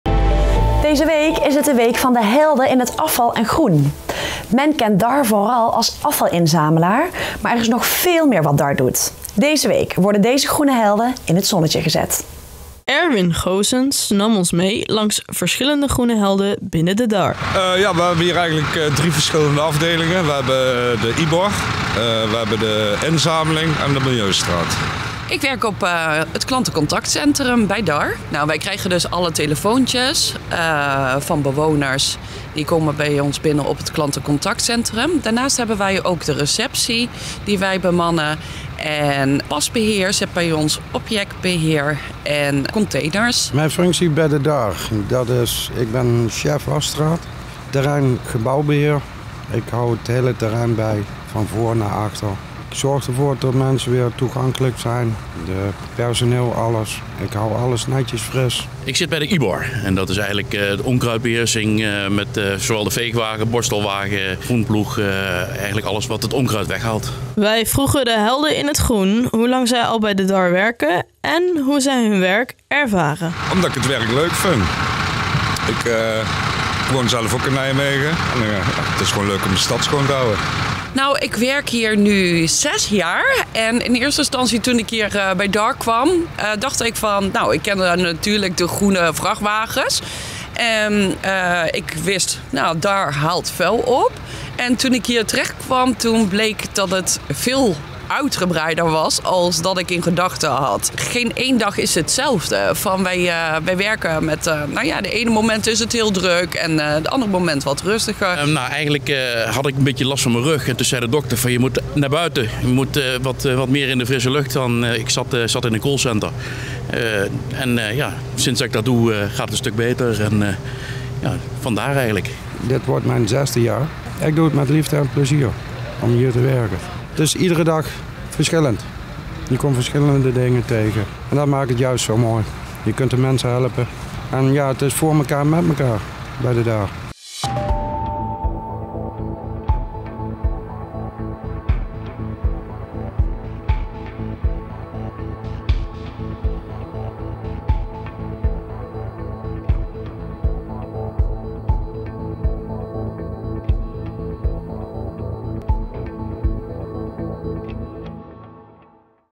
Medewerkers van Dar in het zonnetje gezet || RN7 REGIONIEUWS
Deze week is het de week van de helden in het afval en groen. Men kent Dar vooral als afvalinzamelaar, maar er is nog veel meer wat Dar doet. Deze week worden deze groene helden in het zonnetje gezet.